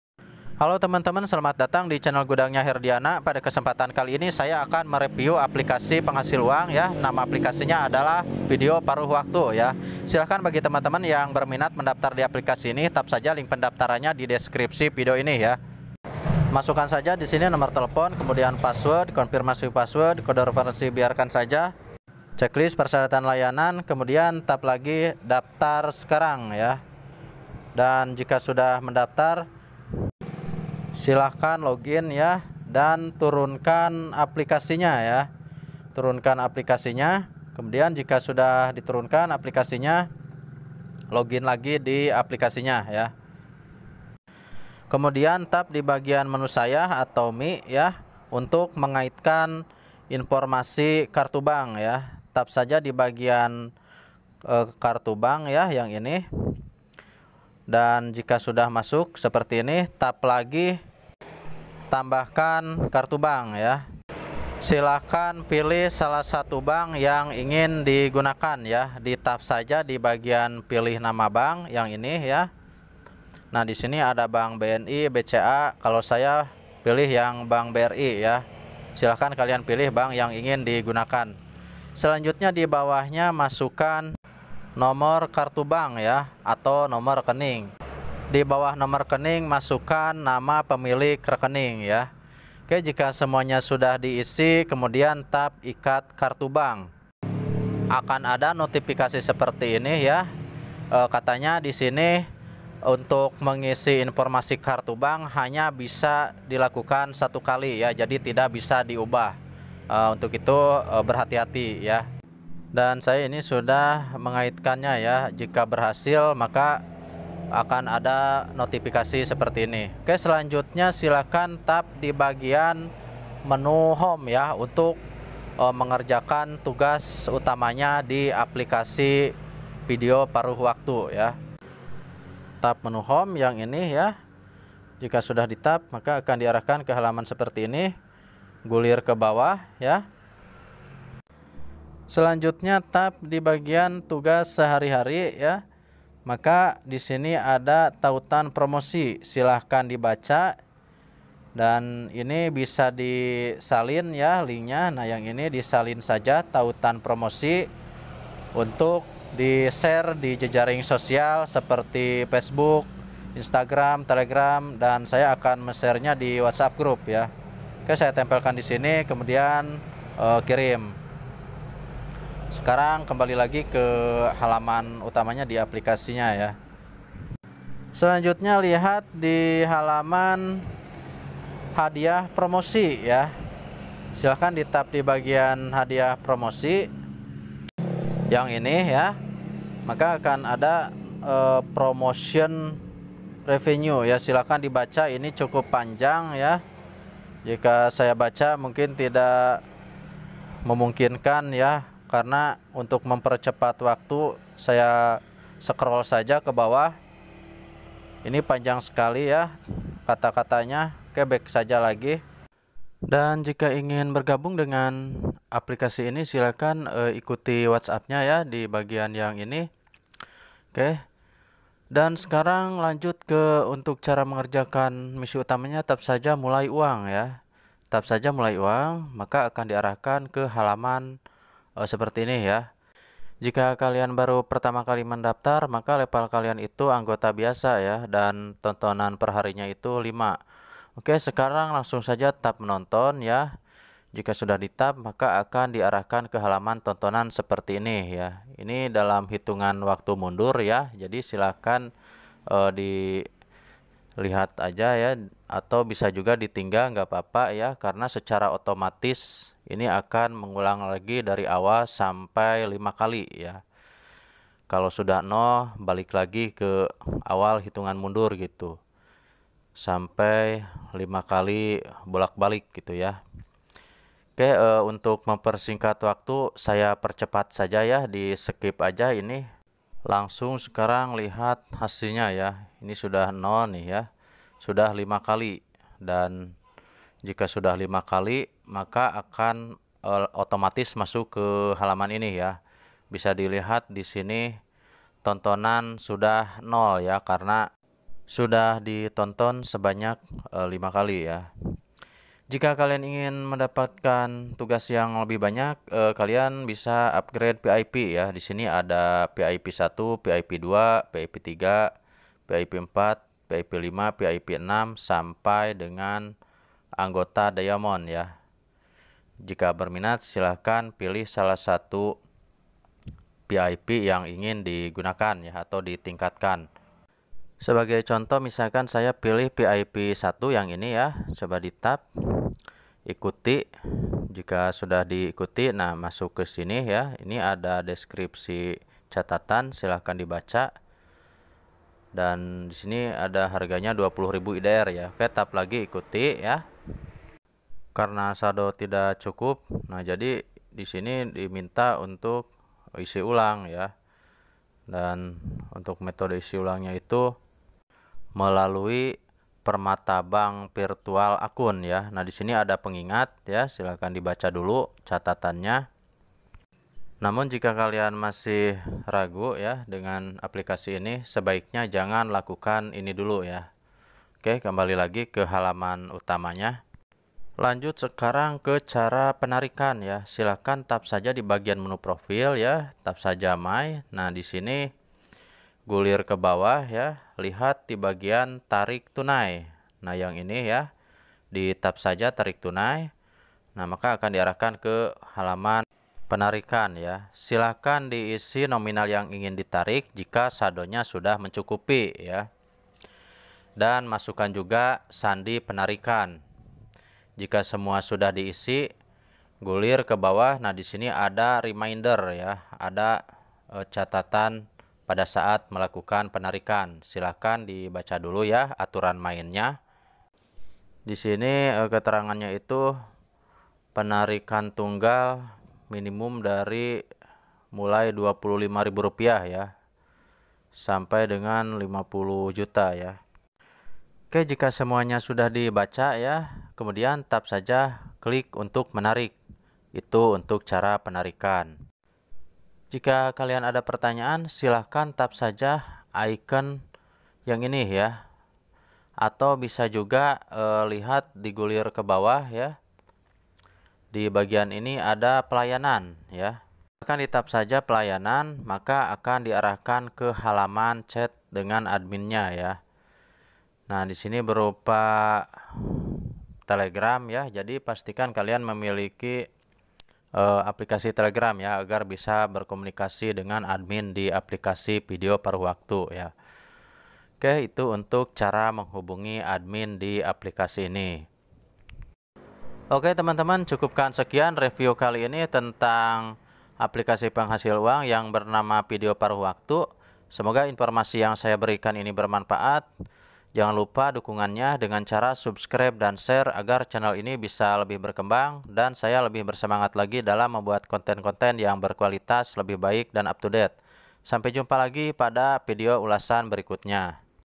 Apk Penghasil Uang Terbaru Video Paruh Waktu, Menjalankan Tugas Utamanya Sangat Mudah Dan Praktis
apk ini merupakan penghasil uang yang cara kerjanya sangat mudah, hanya memutar video singkat yang dihitung dalam waktu mundur.

aplikasi ini sudah scam